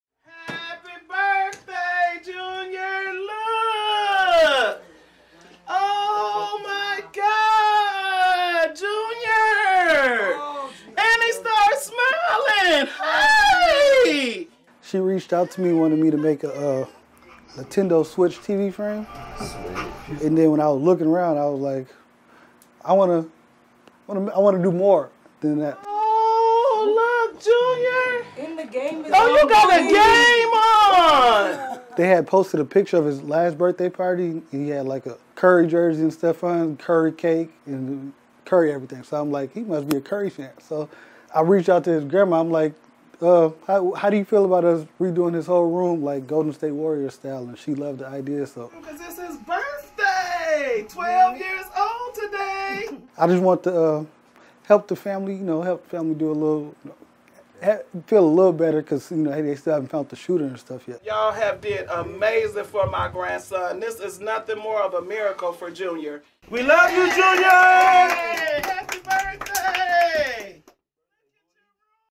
Family, community surprise La'Davionne Garrett, Jr. on his 12th birthday
See the moment La'Davionne Garrett, Jr. was surprised with his new basketball-themed bedroom, as the Minneapolis boy continues to recover from a 2021 shooting.

KARE 11 was there for the big reveal, and spoke to custom woodworker and north Minneapolis native Tyrone Raino, who helped design the room.